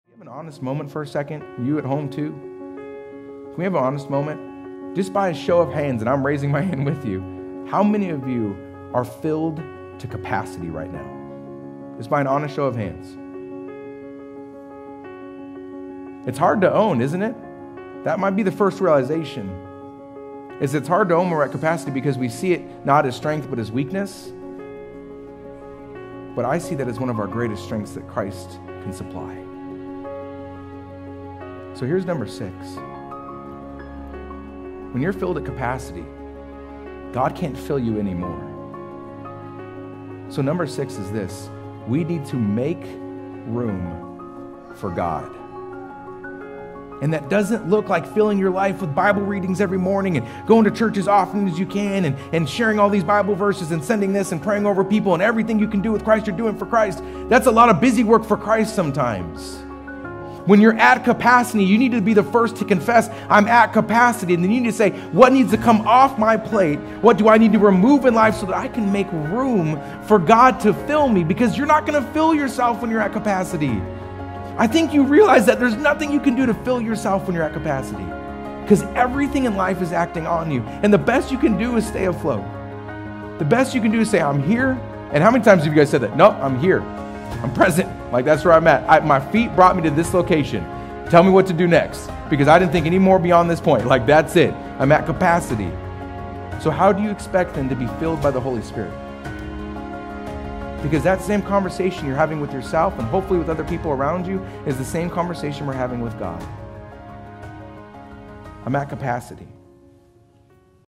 Clip "Make Room for God" (Part 3 - This Year I Will ______ )
We are at capacity. Maxed. Filled up already. No more room. Yet, we are supposed to fill others and for God to fill us. If we have no more room? How can God fill us?